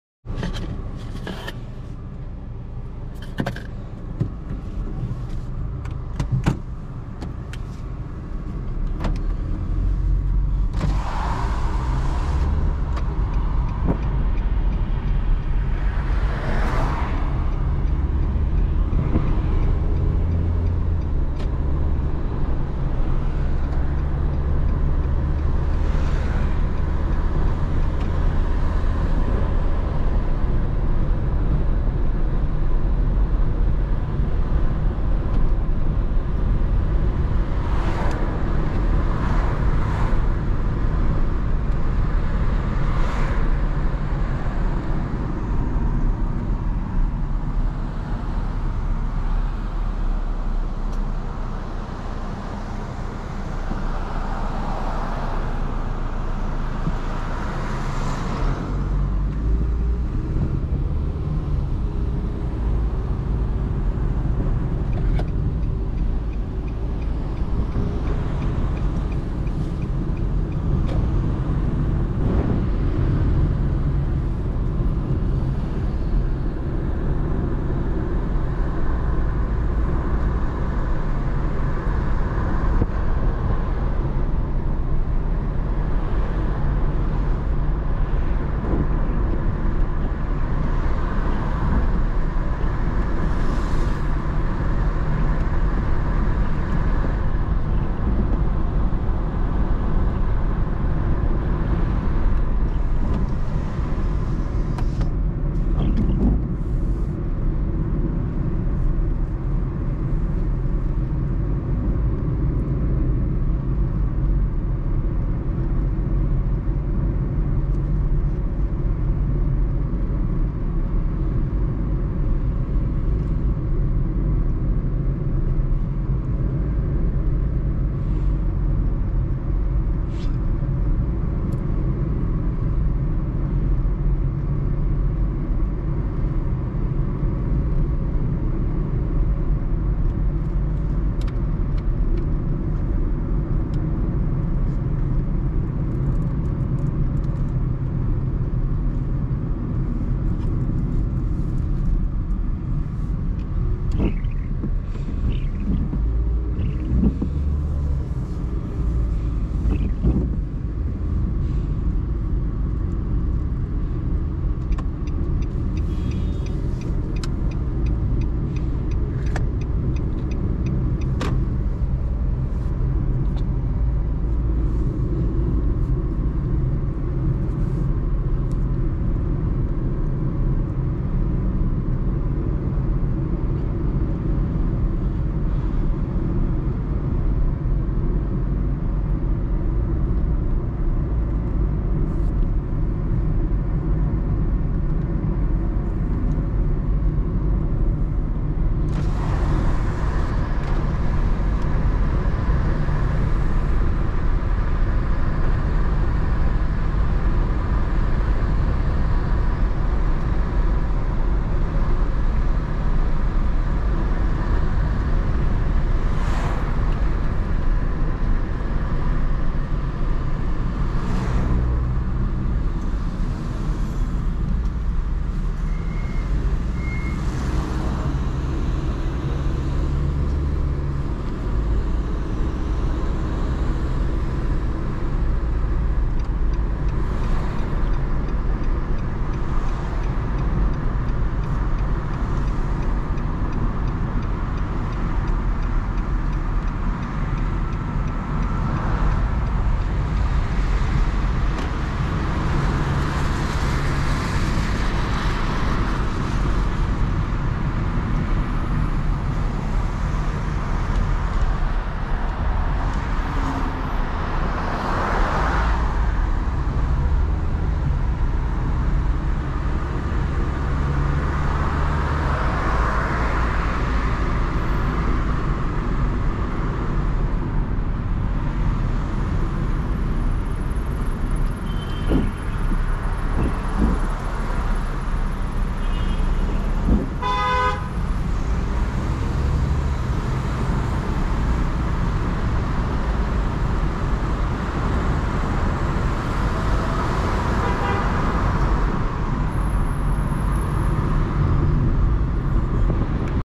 نيسان صني 2022 تجربة قيادة وعزل في المساء
بلد الصنع:اليابان
الموديل: 2022
عدد الركاب: 5
لون المركبة: فضي
نوع المركبة: نيسان صني
1.6 لتر
كم: 43357 كم
V4